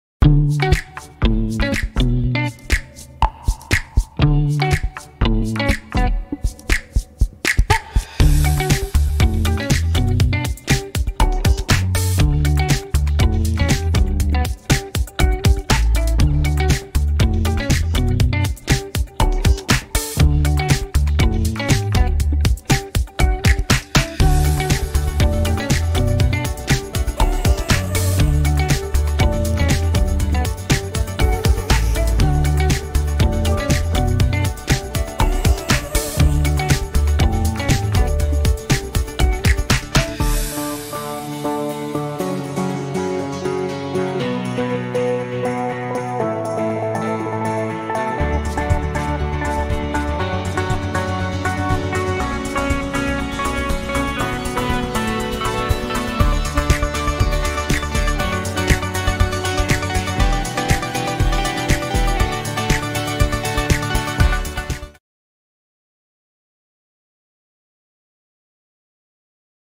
Grafička dizajnerica izrađuje mini jestivu odjeću i njome “odijeva” poznate zvijezde
Grafička dizajnerica Ruby Perman na svojem TikTok profilu objavljuje kako izrađuje mini jestivu odjeću i njome “odijeva” poznate zvijezde.